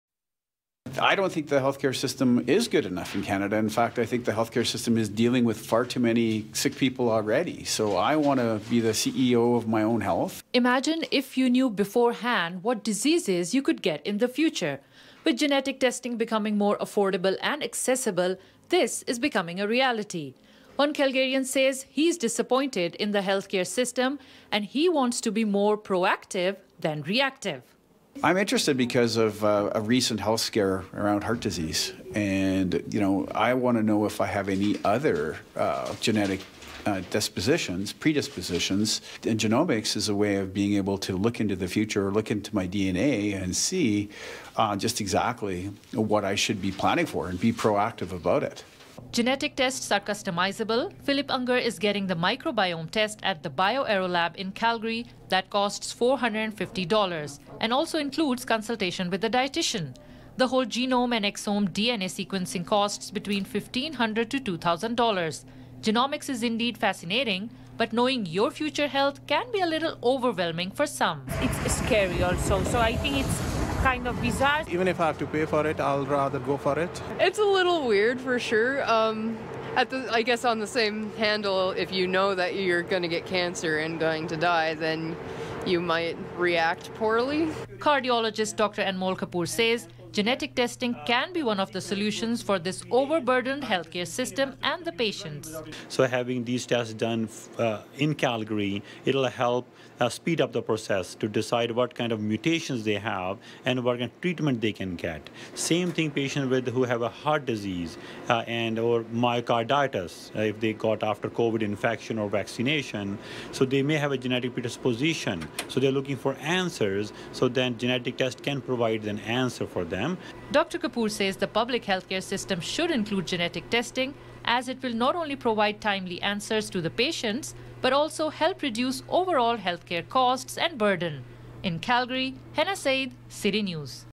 Calgary Resident Takes Charge of Their Health with Genetic Testing from Bioaro
Be the CEO of your health, its in your genes.

BioAro is leading Genomics in Canada and breaking down barriers.